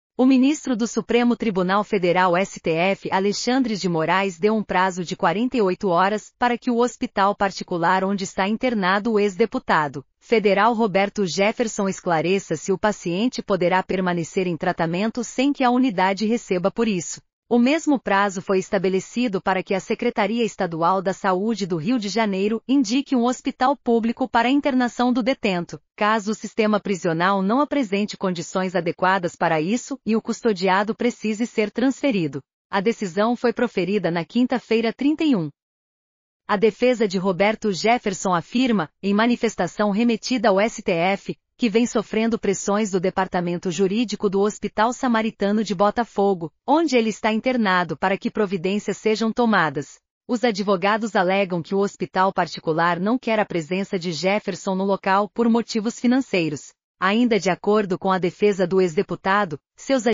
Moraes dá 48 horas para hospital e governo do RJ decidirem sobre transferência de Roberto Jefferson
Defesa do ex-deputado afirmou que vem sofrendo pressões do departamento jurídico do Hospital Samaritano de Botafogo, onde ele está internado, para que providências sejam tomadas